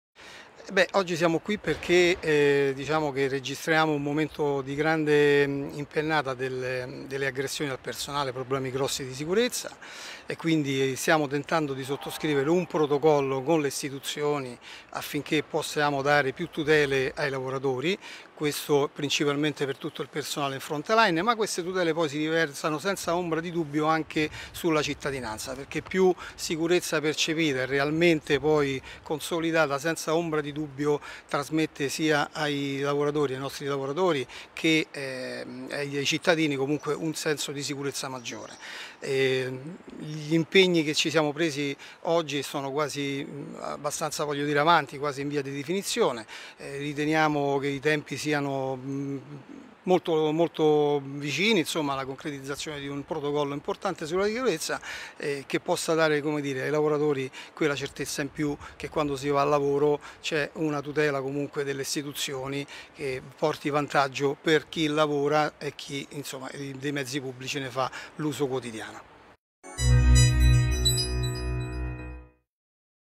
Roma - ESPOSITO (UIL TRASPORTI): IMMINENTE PROTOCOLLO CONTRO AGGRESSIONI AUTISTI BUS (15.04.24)
- Roma - ESPOSITO (UIL TRASPORTI): IMMINENTE PROTOCOLLO CONTRO AGGRESSIONI AUTISTI BUS
"Registriamo un momento di grande impennata delle aggressioni al personale, con problemi grossi di sicurezza e stiamo tendando di sottoscrivere un protocollo con le istituzioni affinchè si possa dare più tutele ai lavoratori. Queste tutele si riverseranno anche sulla cittadinanza perché, più sicurezza percepita e poi consolidata trasmette ai lavoratori e cittadini un senso di sicurezza maggiore. Gli impegni di oggi sono in via di definizione e i tempi per la realizzazione del protocollo sono molto vicini". Lo dichiara all'Agenzia Dire il Segretario regionale UIL trasporti Lazio, Fabio Esposito, al termine del secondo incontro, in Prefettura a Roma, tra le categorie dei trasporti dei sindacati, la Regione, il Comune, Atac e Cotral, per contrastare le aggressioni nei confronti degli autisti dei bus e firmare un protocollo contenente misure di contrasto al fenomeno.  (15.04.24)